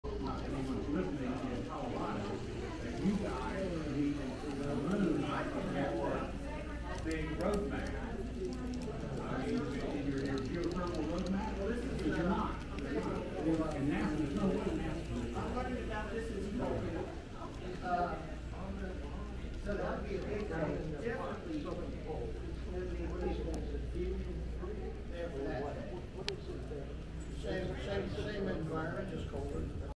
N Scale Trains Running Around The Mountain At The Hickory Train Show 2021
Two very nice N Scale trains running on a modular layout.